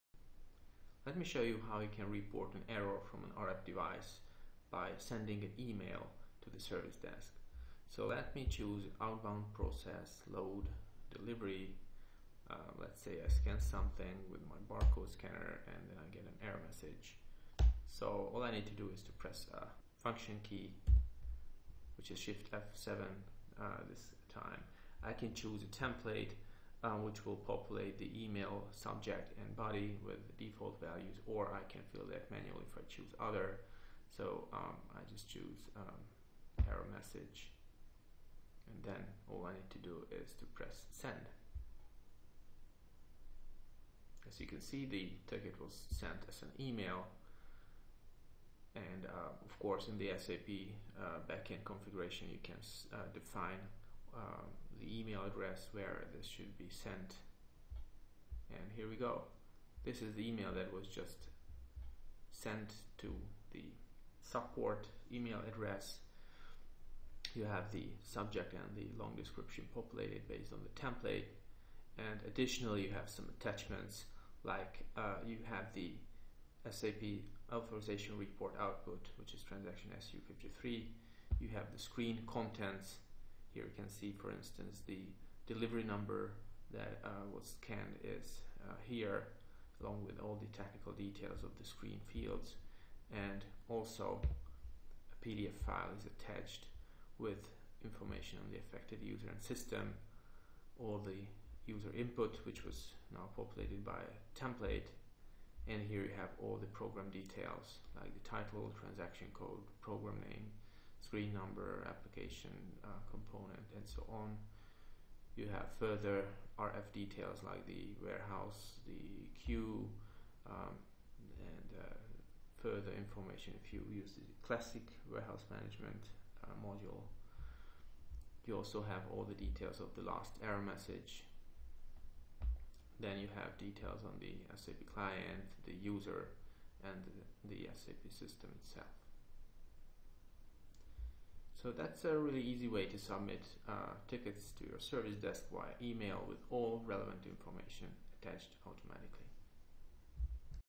Send error reports as email from RF devices running SAP Warehouse Management
Send emails to your service desk directly from RF devices and have all relevant information attached automatically.